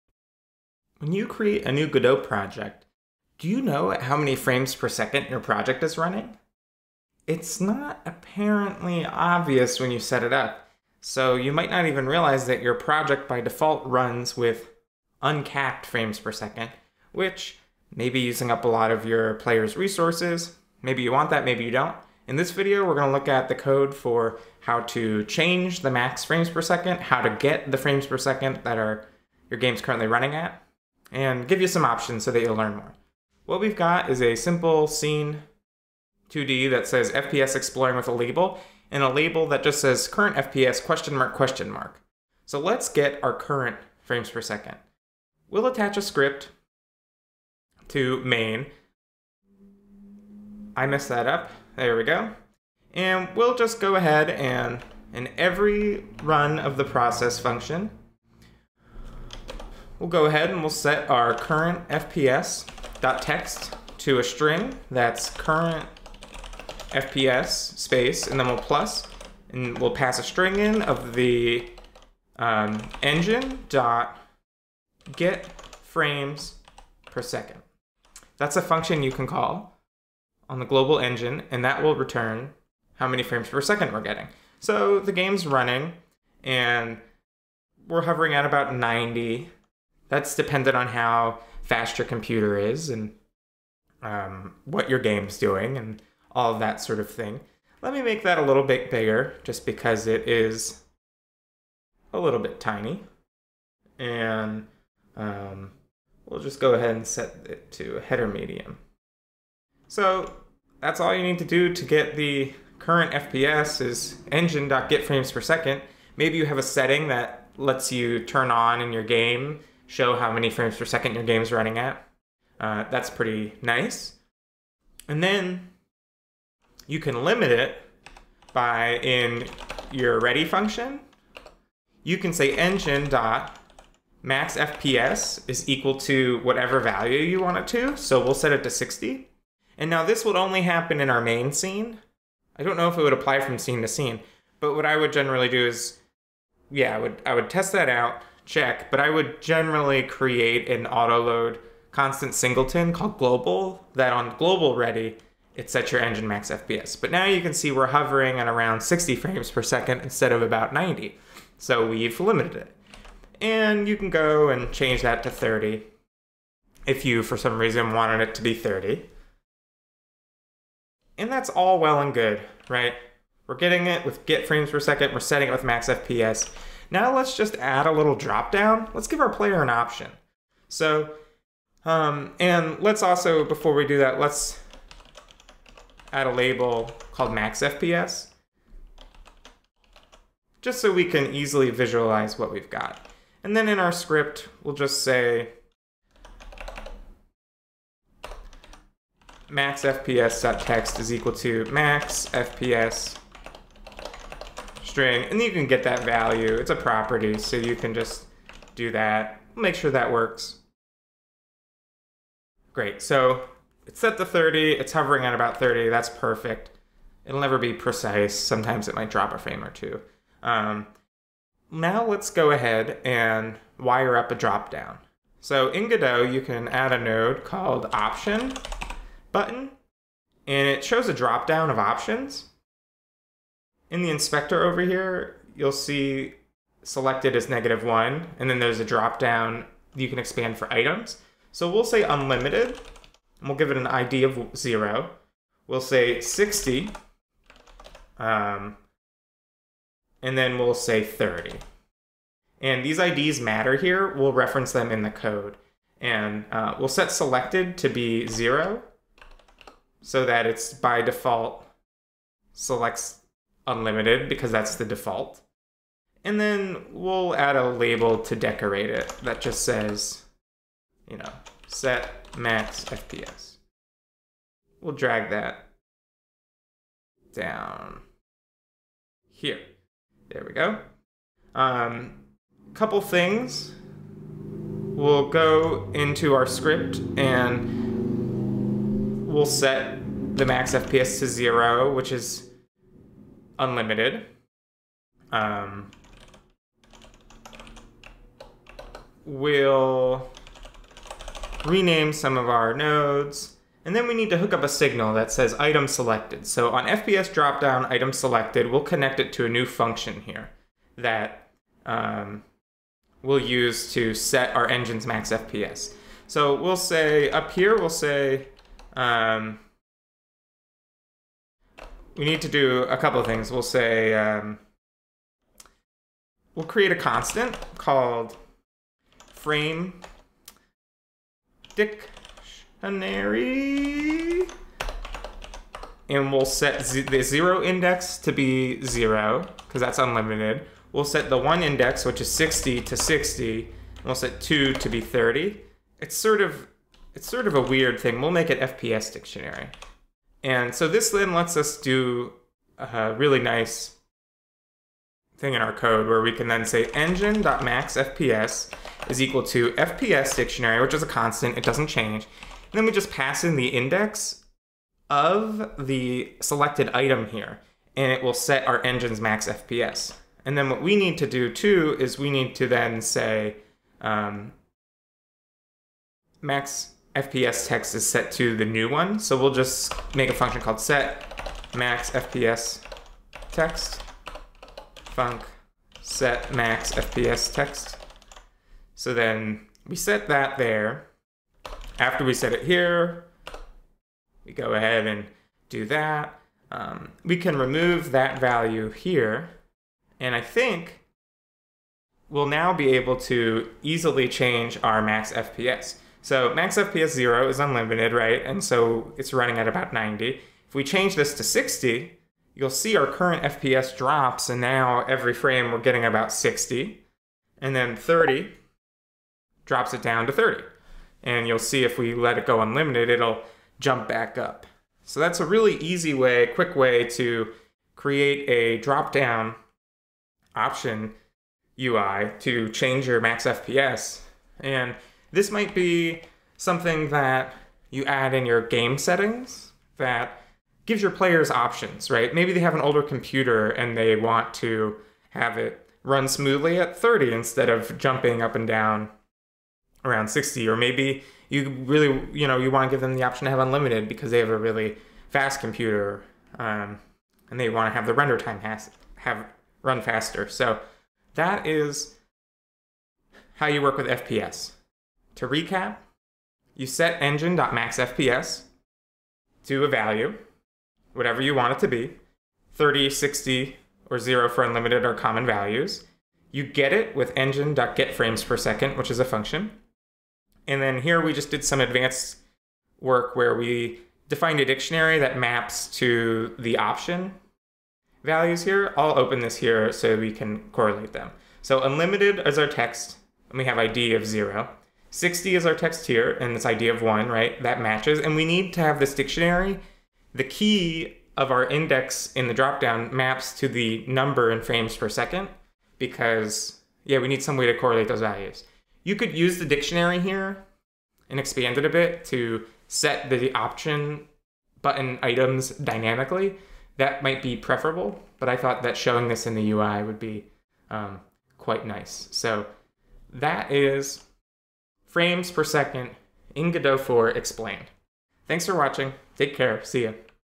How to Get and Limit Frames Per Second in Godot 4
Did you know that new projects in Godot 4 run with an uncapped frame rate? I go over how to set max frames per second (FPS), get the current frames per second, and add a dropdown to change between to give players choice dependent on their needs.

Chapters
00:00 - Intro
00:44 - Get Current FPS
02:07 - Limiting FPS
03:41 - Max FPS Dropdown
07:42 - Recap